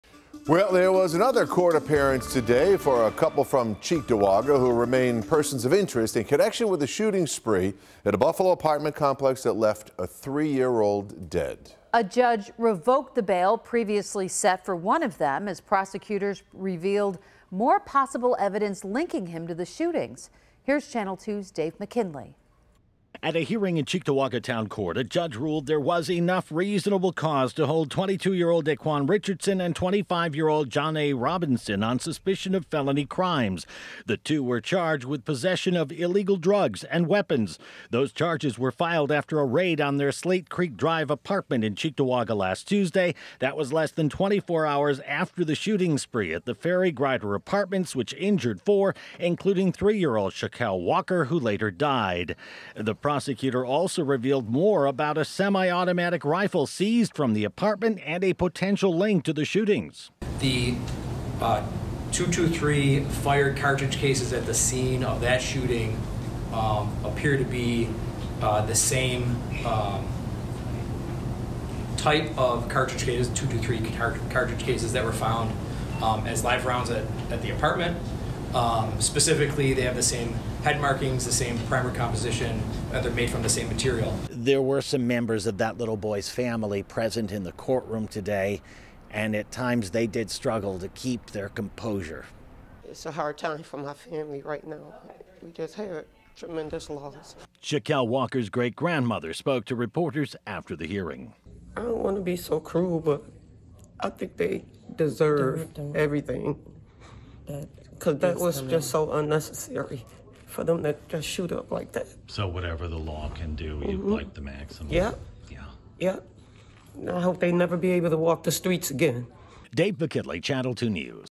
Persons of interest in three year old's shooting
A judge revoked the bail previously set for one of them as prosecutors revealed more about possible evidence linking him to the shooting.